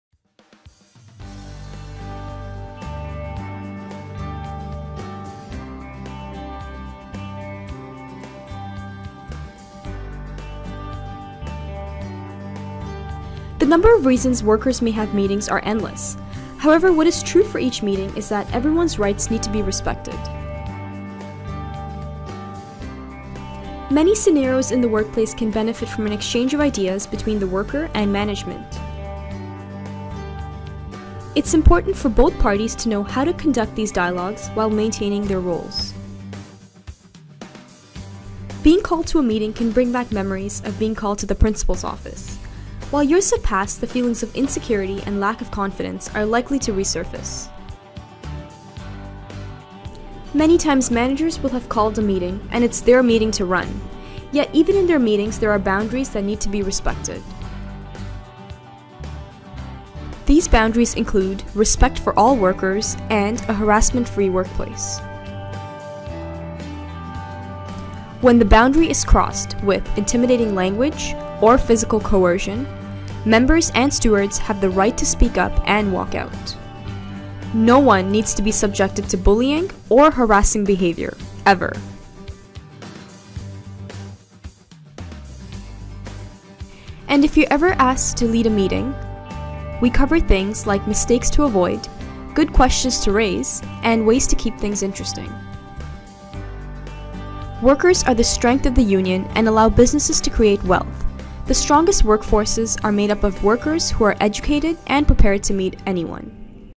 Effective Meetings
A great webCampus course open to members and their families on how to engage in effective meetings.